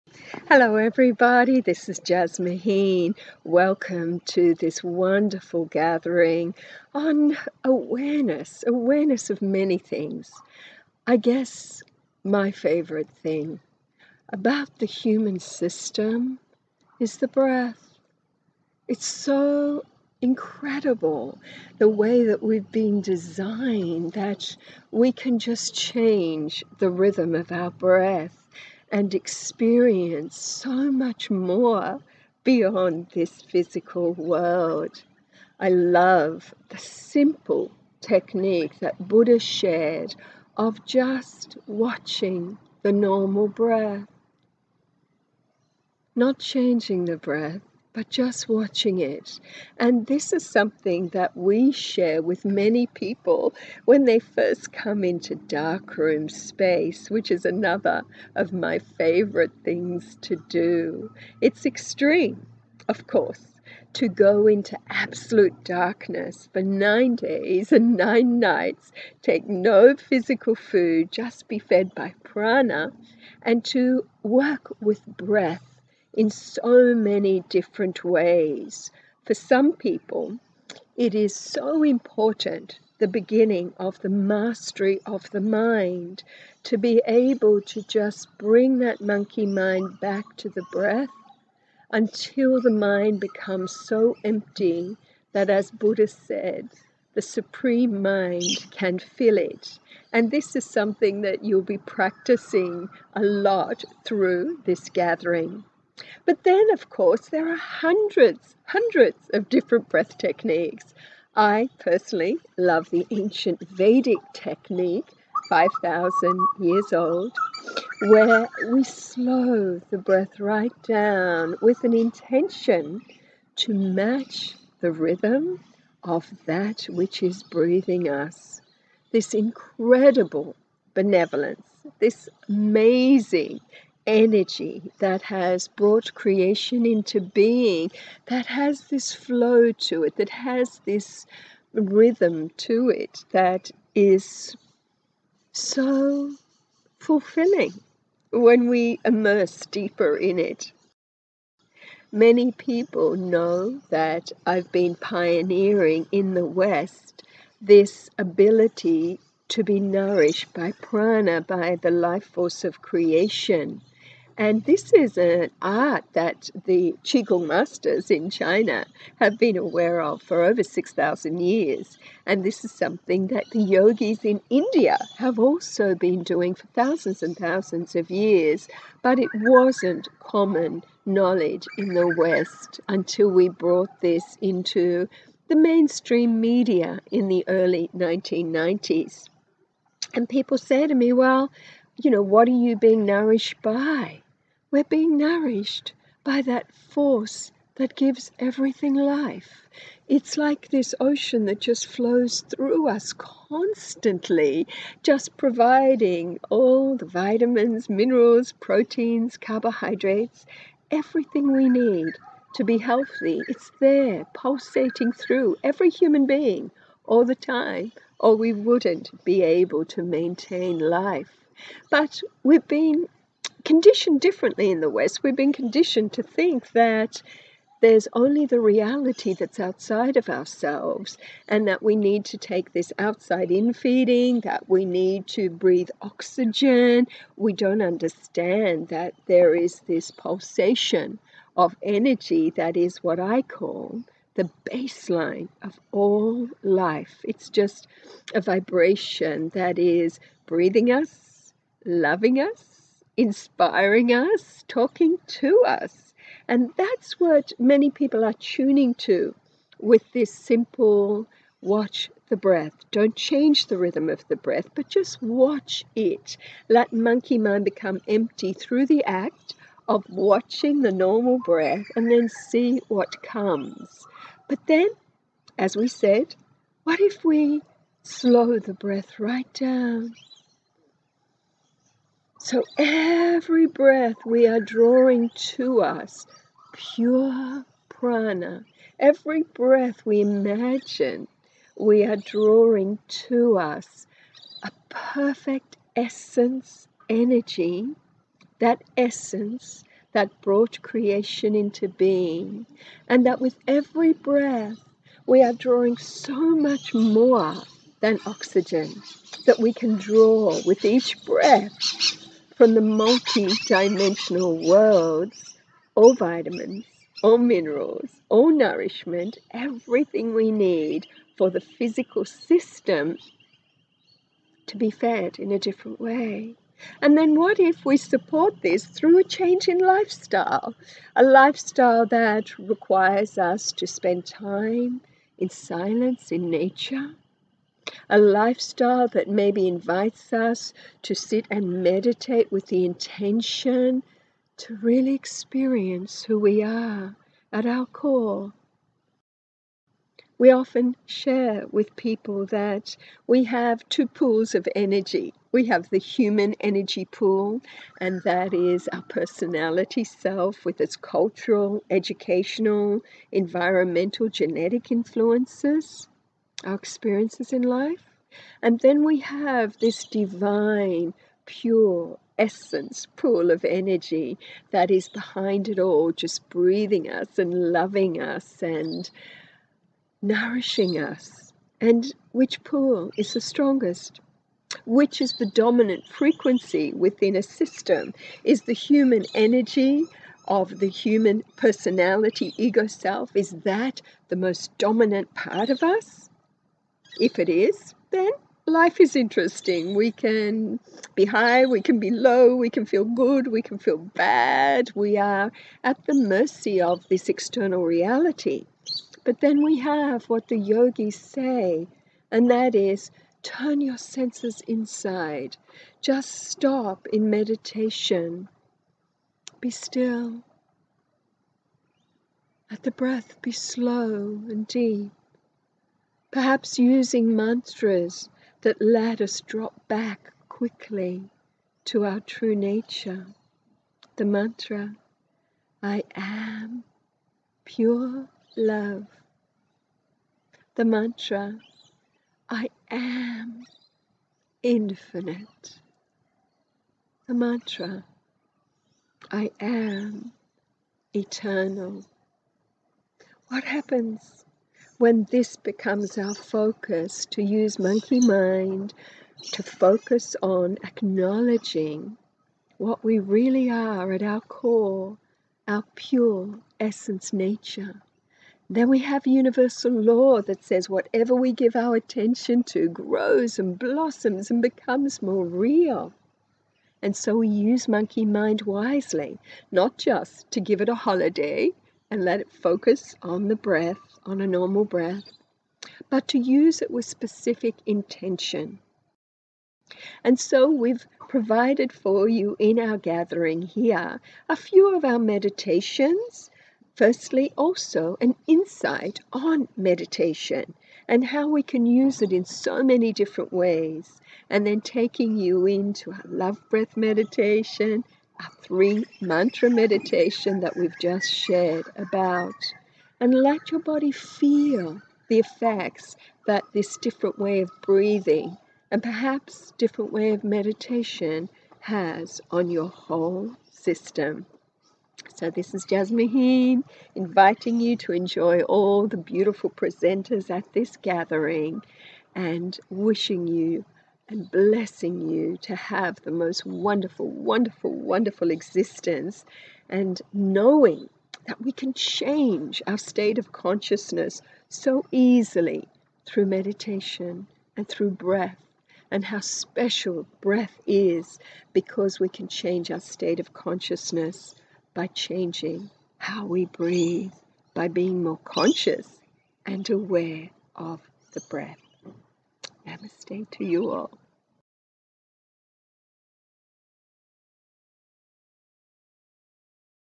Jasmuheen on Breath Awareness, Prana and Meditation
Jasmuheen at her mountain retreat sharing about Breath Awareness, Darkroom, Meditation, Mind Mastery, Pranic living and more - filmed for the Breath Summit Gathering in May 2017.